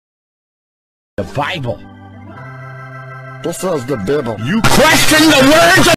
The Bibbl
Sam the demon and Sam the angel arguing

Deviant Art: SilverSmokeTheWolf